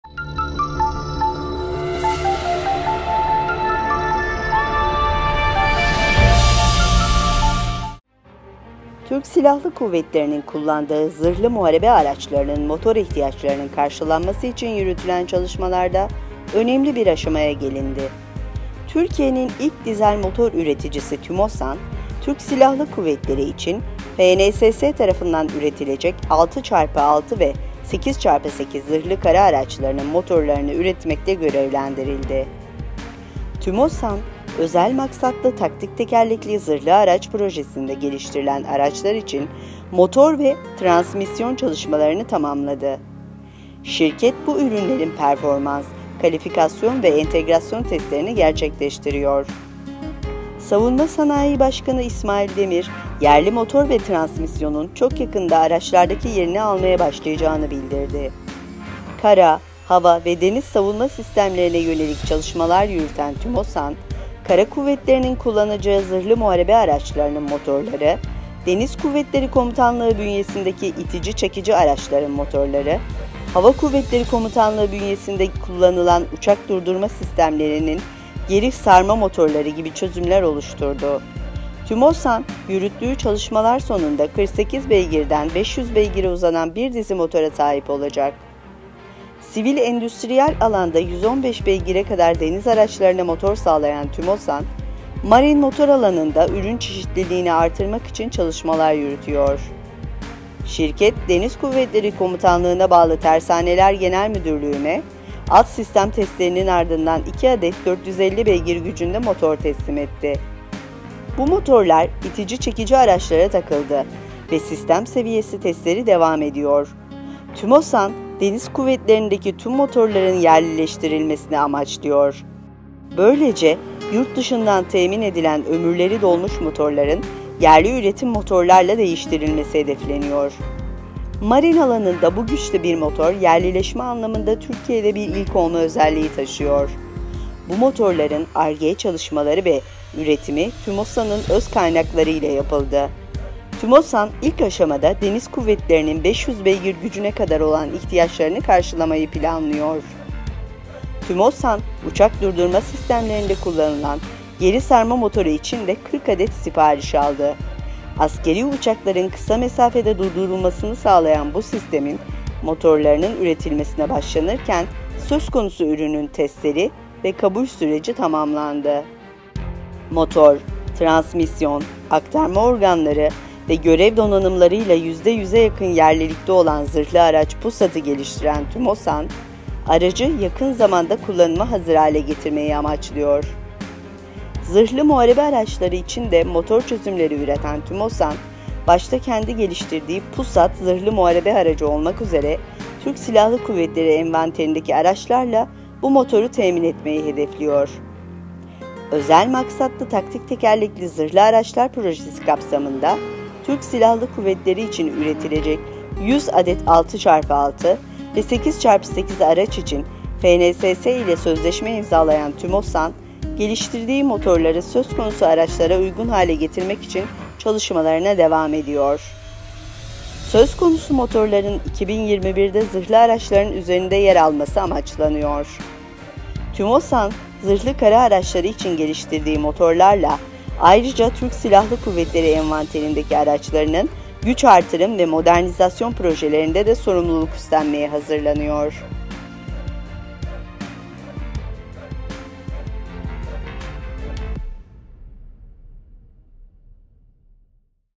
Yerli motor ve transmisyon göreve hazırlanıyor - TÜMOSAN - FNSS - PARS 6X6 - PARS 8X8 - TMSN
Türk Silahlı Kuvvetlerinin kullandığı zırhlı muharebe araçlarının motor ihtiyaçlarının karşılanması için yürütülen çalışmalarda önemli bir aşamaya geldi.

Türkiye'nin ilk dizel motor üreticisi TÜMOSAN, Türk Silahlı Kuvvetleri için FNSS tarafından üretilecek 6x6 ve 8x8 zırhlı kara araçlarınının motorlarını üretmekle görevlendirildi.

TÜMOSAN, Özel Maksatlı Taktik Tekerlekli Zırhlı Araç Projesinde geliştirilen araçlar için motor ve transmisyon çalışmalarını tamamladı. Şirket, bu ürünlerin performans, kalifikasyon ve entegrasyon testlerini gerçekleştiriyor. Savunma Sanayii Başkanı İsmail Demir, yerli motor ve transmisyonun çok yakında araçlardaki yerini almaya başlayacağını bildirdi.

Özel Maksatlı Taktik Tekerlekli Zırhlı Araçlar Projesi kapsamında Türk Silahlı Kuvvetleri için üretilecek 100 adet 6x6 ve 8x8 araç için FNSS ile sözleşme imzalayan TÜMOSAN, geliştirdiği  motorları söz konusu araçlara uygun hale getirmek için çalışmalarına devam ediyor. Söz konusu motorların 2021’de zırhlı araçların üzerinde yer alması amaçlanıyor.

The work carried out to meet the engine needs of armored combat vehicles used by the Turkish Armed Forces has reached an important stage.

FNSS will produce 6x6 and 8x8 armored vehicles for the Turkish army. Turkey's first diesel engine manufacturer TÜMOSAN was tasked with producing the engines of these vehicles.

TÜMOSAN has completed the engine and transmission studies for the vehicles developed in the Special Purpose Tactical Wheeled Armored Vehicle Project. The company performs performance, qualification and integration tests of these products. Defense Industry President İsmail Demir stated that the domestic engine and transmission will start to take its place in vehicles very soon.

TÜMOSAN, which signed a contract with FNSS for 100 Pars Scout 6x6 and Pars III 8x8 vehicles to be produced for the Turkish Armed Forces with the Special Purpose Tactical Wheeled Armored Vehicles Project, continues to work to make the engines it developed suitable for these vehicles. The engines in question are intended to be placed on armored vehicles next year.